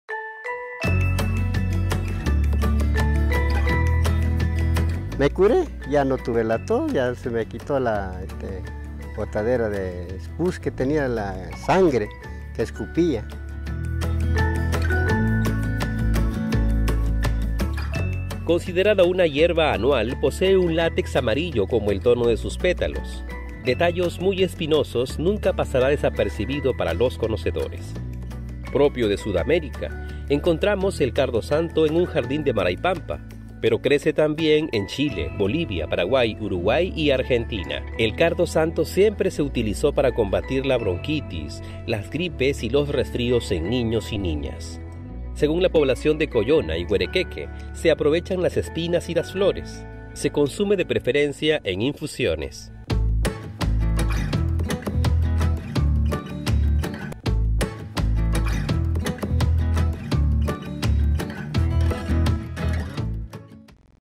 Cardo Santo - planta medicinal
Conoce las propiedades medicinales de la planta: "Cardo santo".